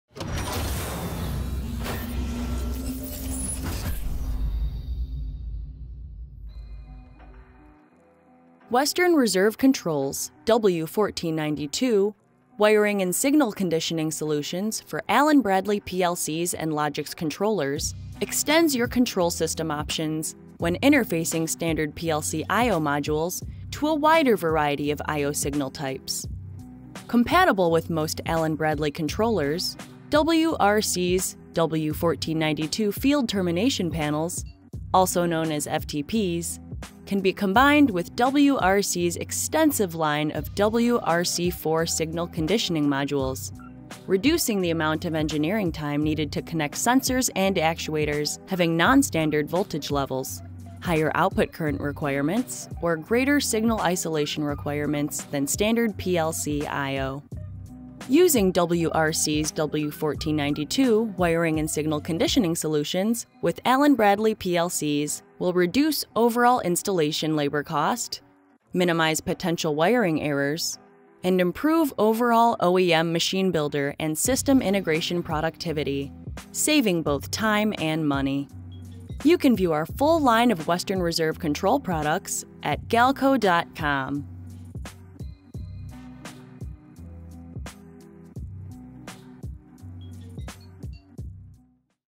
Western Reserve Controls W1492 Wiring and Signal Conditioning Solutions for Allen-Bradley PLCs
Western Reserve Controls W1492 Wiring and Signal Conditioning Solutions for Allen-Bradley PLCs and LOGIX Controllers extend your control system options when interfacing standard PLC I/O modules to a wider variety of I/O signal types.